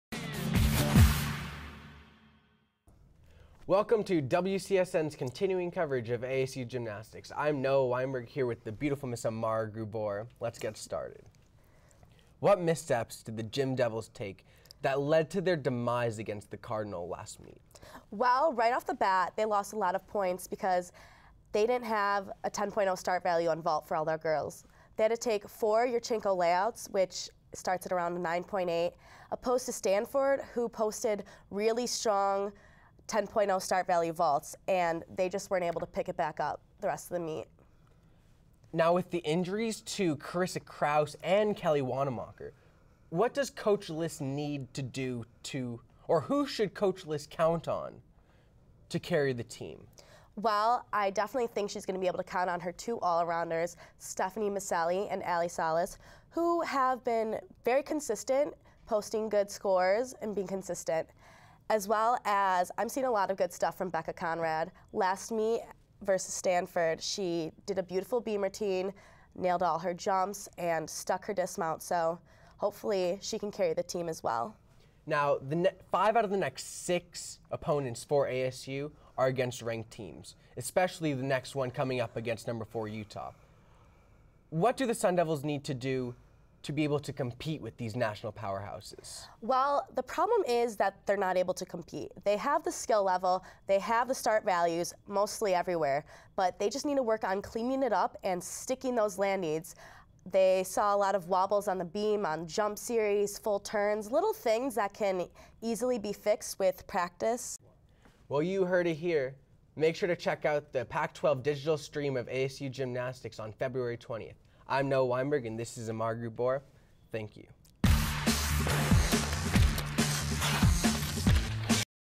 ASU Gymnastics: Moving forward in the season
ASU gymnastics analysts Noah Weinberg and Amara Grubor break down what the Gym Devils have done so far and what fans can expect from them in the upcoming weeks.